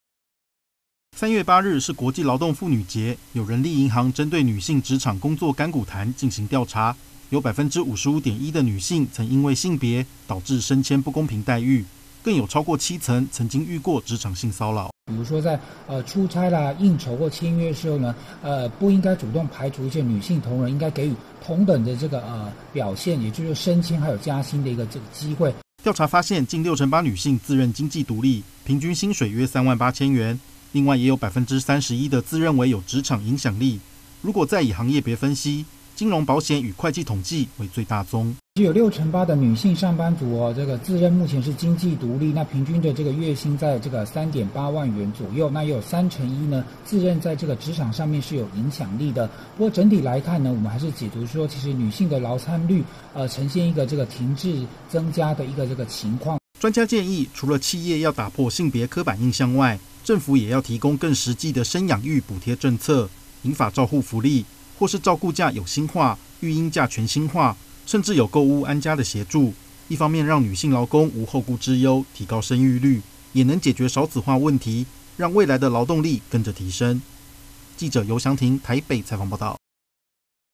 職業婦女壓力大 性別歧視.性騷擾比例高
3月8日婦女節，人力銀行特別公佈「職場女力崛起與工作甘苦談調查」，發現女性不只應徵時遭到歧視，還有55.1%的女性，曾在薪資與升遷上遇到差別對待，甚至有超過7成的婦女表示，遇過職場性騷擾。